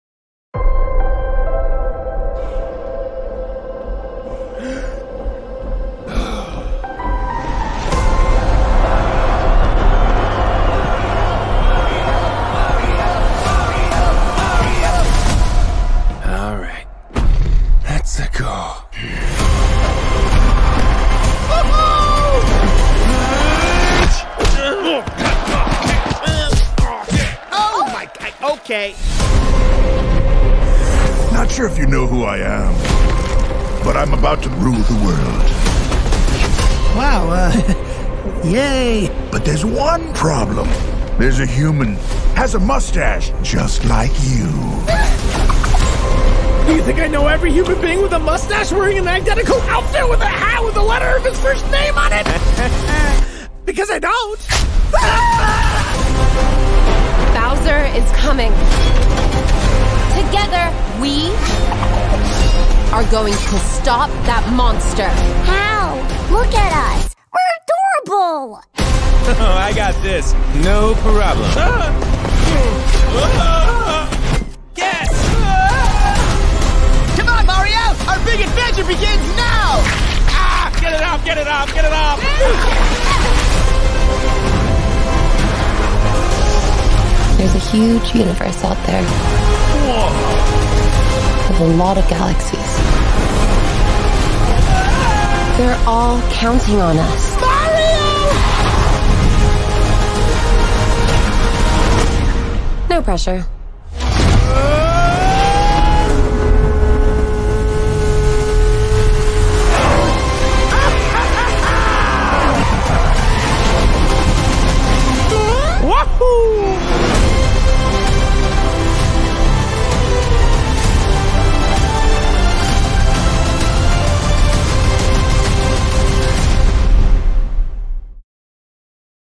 The Super Davi Sibs. Movie Official Trailer
Here it is everyone! My official trailer of the Super Davi Sibs. Sorry it took so long to make, but, it's finally here, I hope you guys enjoy this special trailer, featuring  @davicrossoverproducoes4641   as Super Mario!

P.S: Ginormica is still my main Rascalsona, just using Mirabel for this spoof.